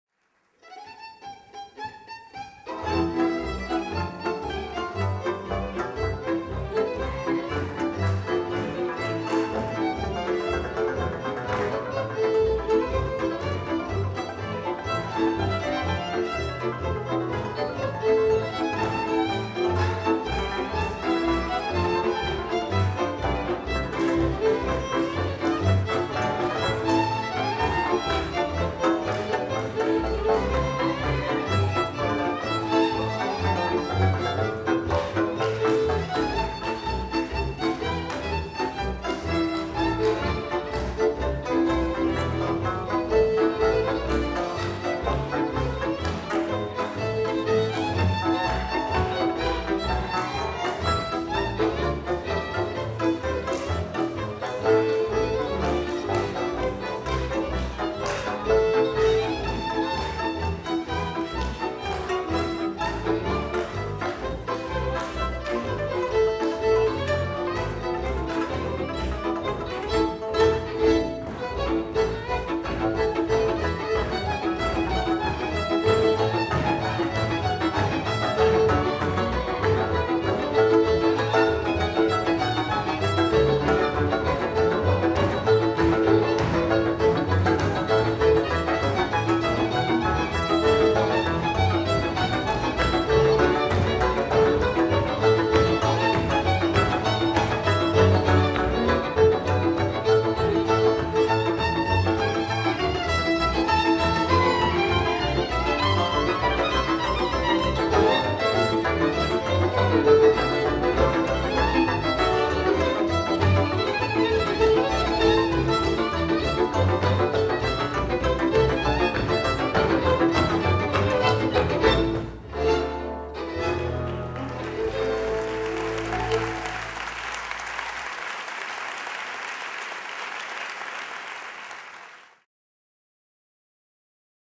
Lőrincz Hortenzia - (Kovács Norbert), Kurucz Kata - (Tókos Attila): Dél-alföldi oláhos és frisses
21. Országos Szólótáncfesztivál
2013. február 22-24.  Békéscsaba
Csabagyöngye Kulturális Központ

Kötelező tánc:  Dél-alföldi oláhos és frisses

 Lőrincz Hortenzia - (Kovács Norbert)
 Kurucz Kata - (Tókos Attila)

 Kísér: Dűvő Zenekar